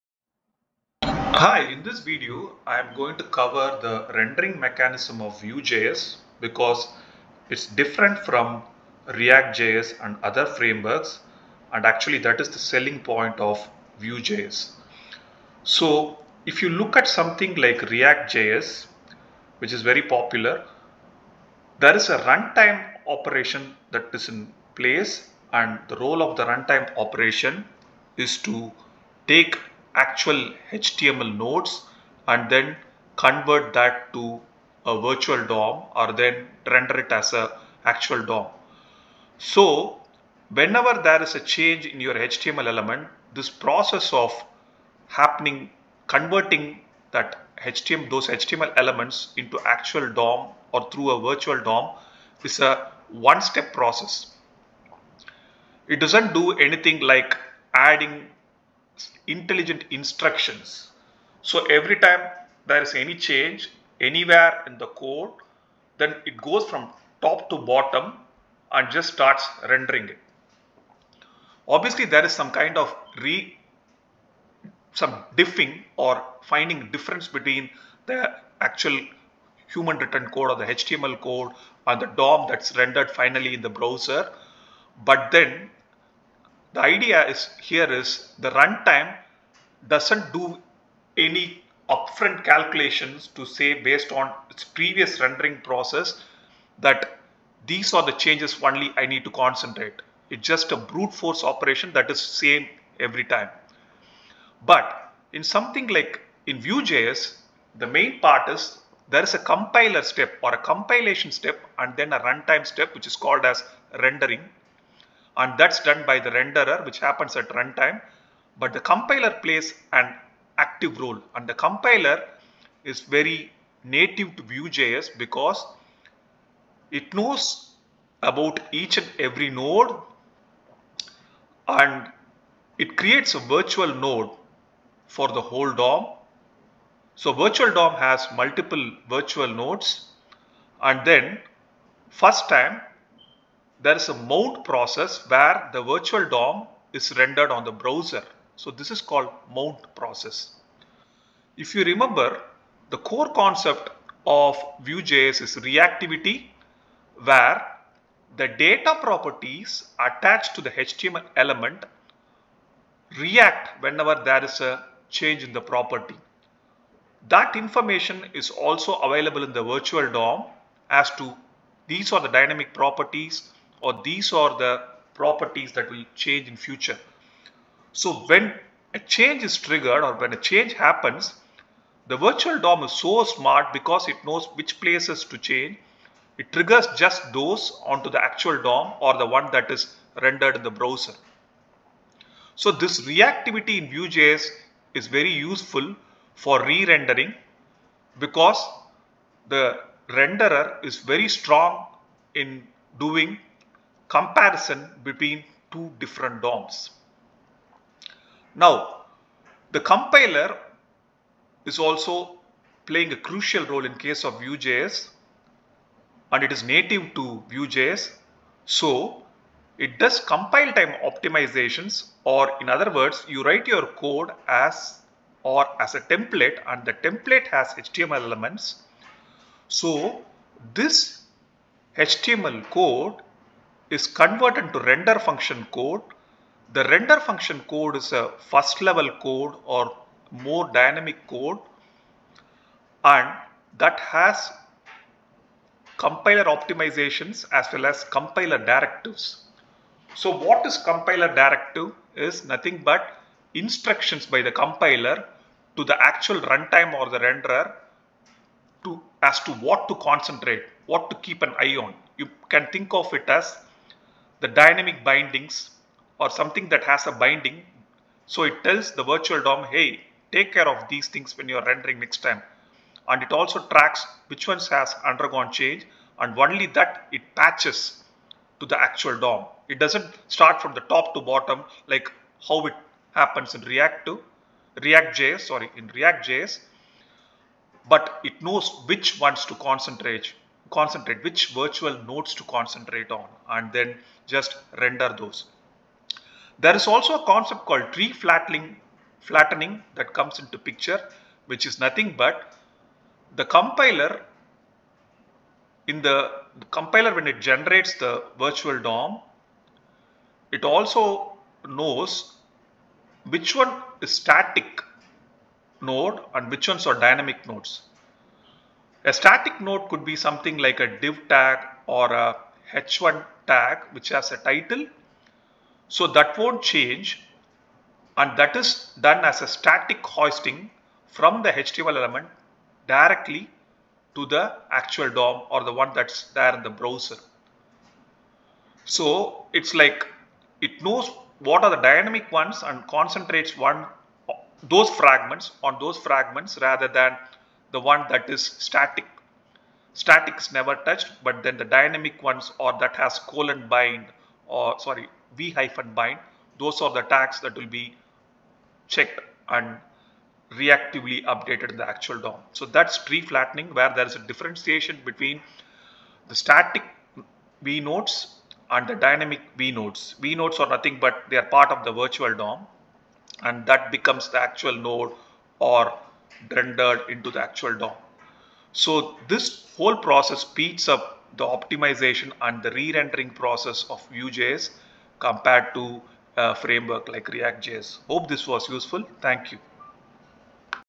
VueJS under 5: Vue JS rendering mechanism
How is re-rendering in vue js better than react js... react js is based on brute-force runtime rendering while vue js rendering mechanism banks on compiler directives.